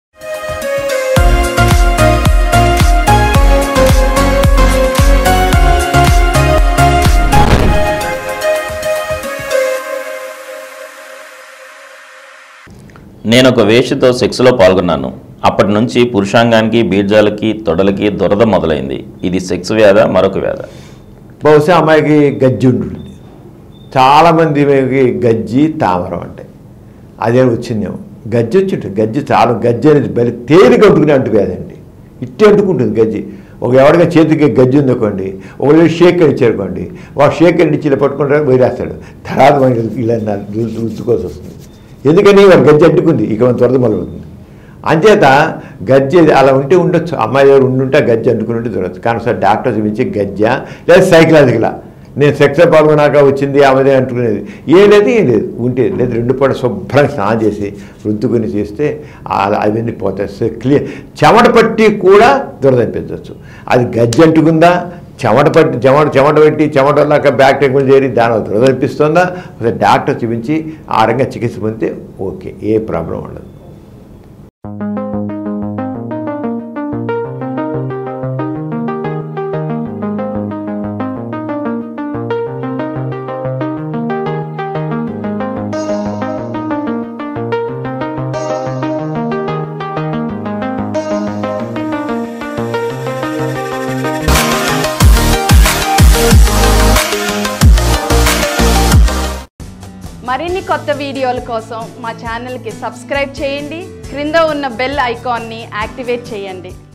వేశ్యను కలవడం వల్లే ఇలా అయిందంటారా? Dr Samaram Mee Kosam (Question 158)
Dr Samaram answering the Romance, Health and Marital problems in Telugu in Dr Samaram Mee Kosam Channel.